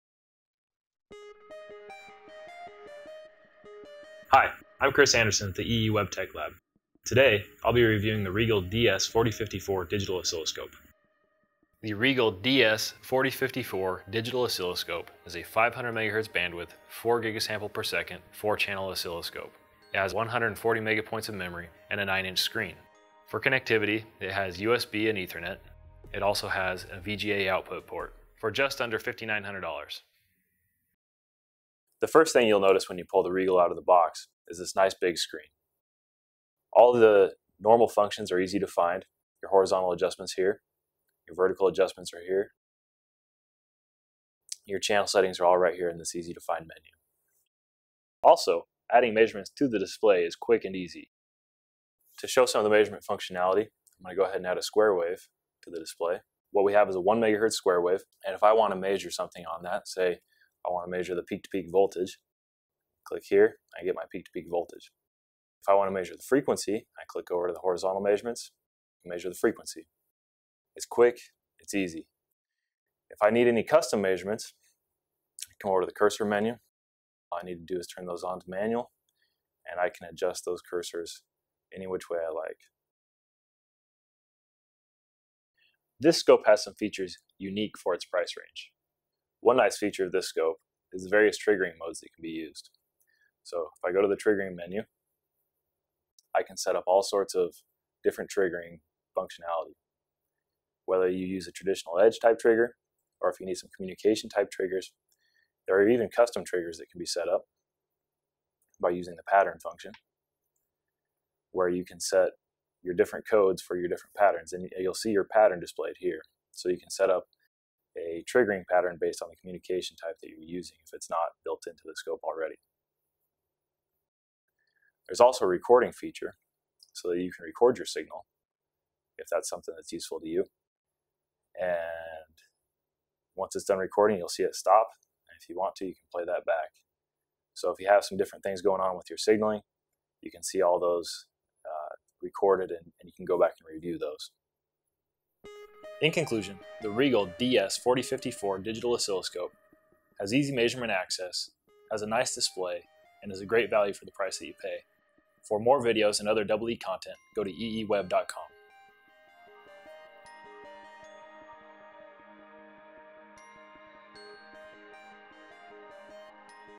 RIGOL DS-4054 Digital Oscilloscope - Product Overview
A professional user guide to help you get started testing in little to no time.